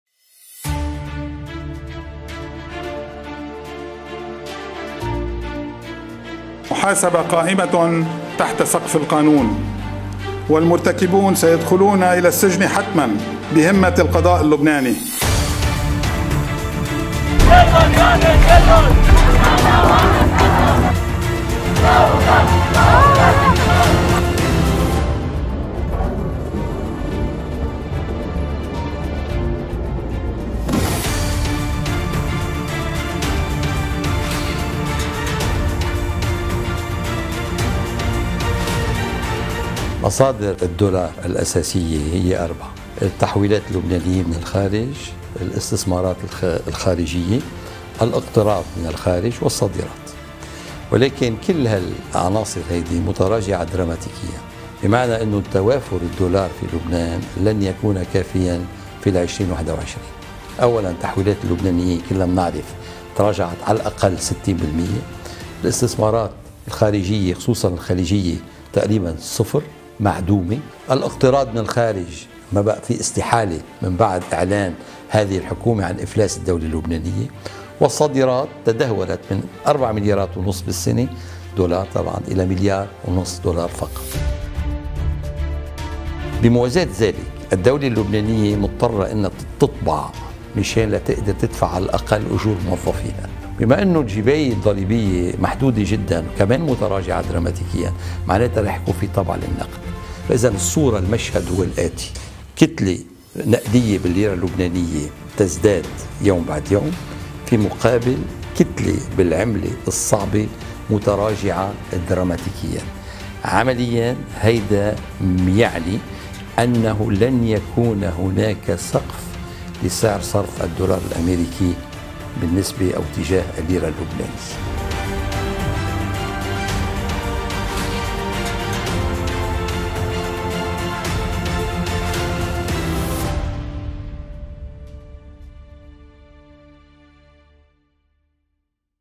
الدولار في 2021... خبرٌ "محزن" لـ اللبنانيين!
لا شك أن العام 2020 كان كارثيًا على عدة مستويات.

وبإستثناء جريمة إنفجار مرفأ بيروت المروّعة التي وقعت في 4 آب، فإن إرتفاع سعر الدولار مقابل الليرة كان ولا يزال أكثر ما أضر باللبنانيين.

ما هو السيناريو المتوقع لسعر الدولار في 2021؟، وهل من معجزة إلهية تُخرج لبنان من الظلمات إلى النور؟.